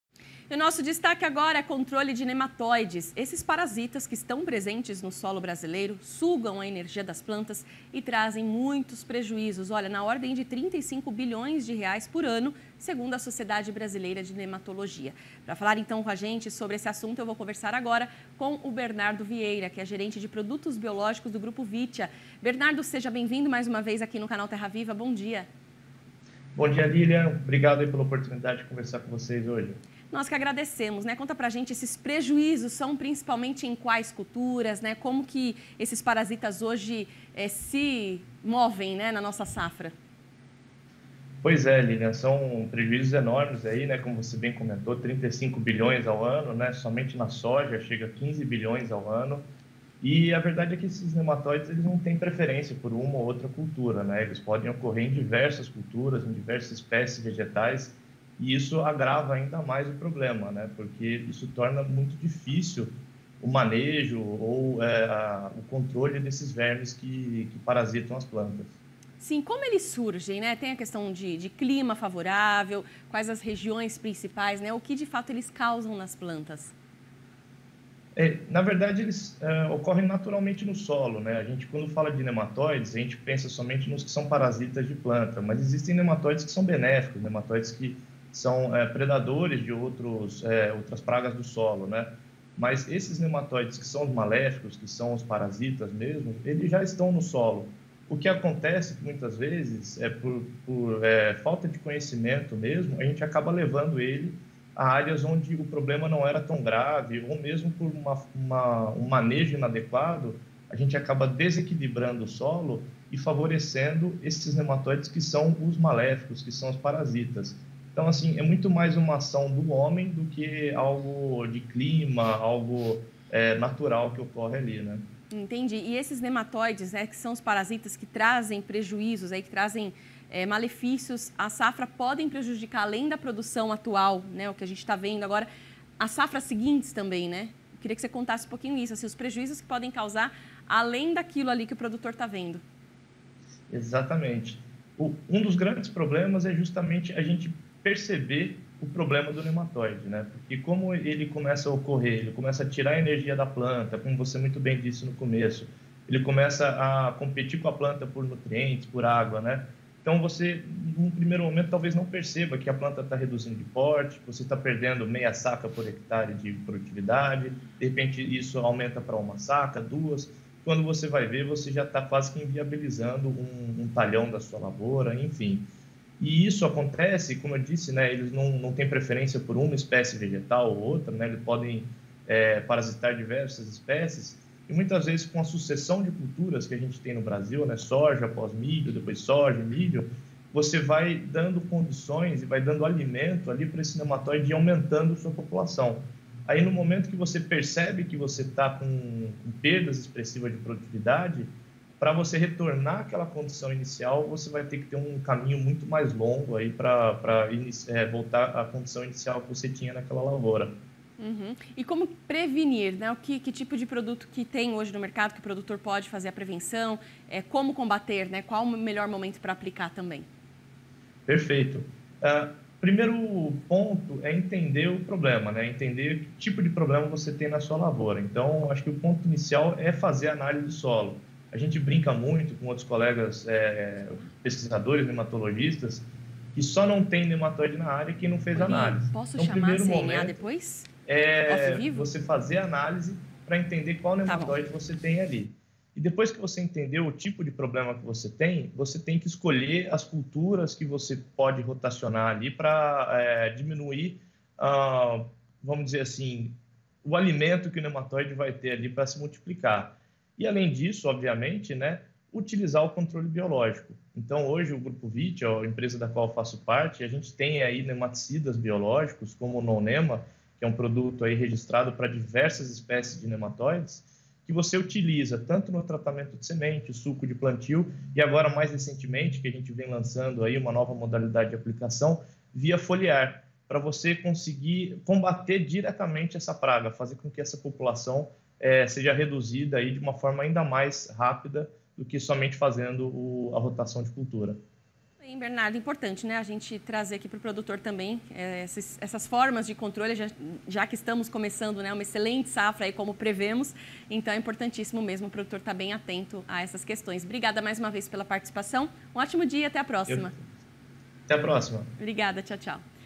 Nematoides: parasitas presentes no solo trazem prejuízos na ordem de 35 bilhões de reais por ano
Os nematoides são parasitas presentes no solo brasileiro que sugam a energia das plantas e trazem prejuízos na ordem de 35 bilhões de reais por ano segundo a Sociedade Brasileira de Nematologia. Para falar sobre isso, Lilian Munhoz conversou com o Bernardo Vieira, gerente de produtos biológicos do Grupo Vittia.